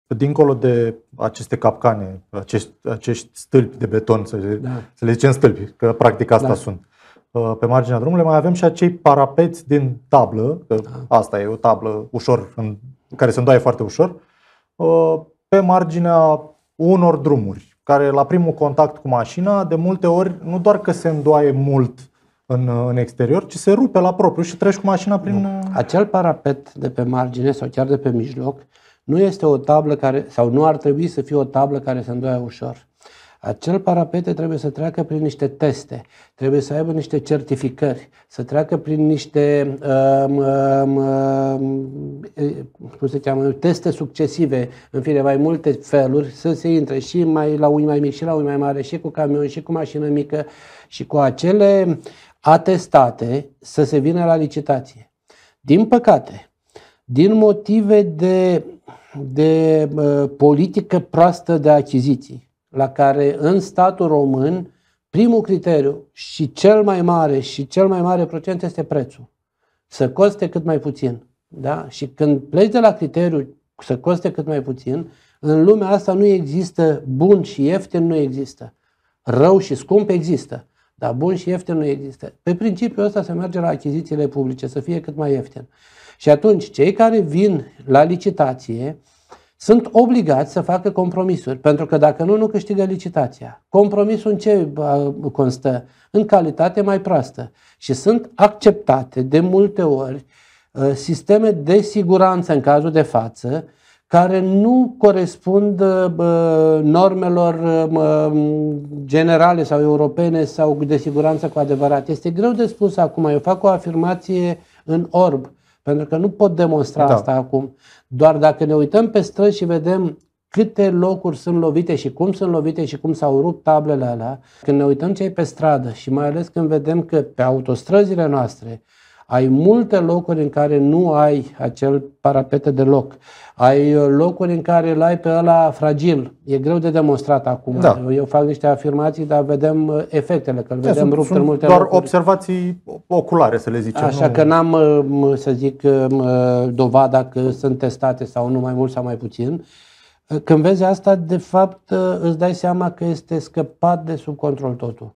Ai încredere în parapetul de pe marginea drumului? Expert: Pe lumea asta nu există bun și ieftin
Fostul pilot de raliuri Titi Aur a explica la DC News TV de ce suntem puși în pericol în mod activ pe străzile din România, din cauza politicilor de achiziții. La noi în țară, cel mai ieftin proiect câștigă licitațiile, iar acest lucru periclitează viața șoferilor.

În cazul unui accident, nu ai siguranța că parapetele de pe marginea șoselei îți va opri mașina sau vei ajunge în râpă.

Nu există lucru bun și ieftin

„Dincolo de stâlpii de beton de pe marginea drumului avem și acele parapete din tablă. La primul contact cu mașina, nu doar că se îndoaie, ci se rup cu totul”, a spus Florin Răvdan.

„Acel parapet de pe margine nu ar trebui să fie o tablă care se îndoaie ușor. Trebuie să treacă prin niște teste, trebuie să aibă niște certificări, să fie testat succesiv. Cu acele atestate ar trebui să se vină la licitație.

Din păcate, din motive de politică proastă de achiziții în care primul criteriu și cel mai important este prețul, nu sunt bune. Când pleci de la acest criteriu, pe lumea asta nu există bun și ieftin. Pe principiul ăsta se merge la achizițiile publice, așa că cei care vin la licitație sunt obligați să facă compromisuri. Sunt acceptate de multe ori sisteme de siguranță în cazul de față care nu corespund normelor generale sau europene”, a explicat Titi Aur.

Situația e scăpată de sub control

„Este greu de spus acum. Eu fac o afirmație în orb, pentru că nu pot demonstra asta acum. Dar dacă ne uităm pe străzi cum se rup tablele alea... Mai ales când vedem că pe autostrăzile noastre ai multe lucruri în care nu ai acel parapete deloc sau pe cel fragil. Nu am dovada acum că sunt testate sau nu, dar vedem asta și ne dăm seama că totul e scăpat de sub control”, a conchis expertul auto.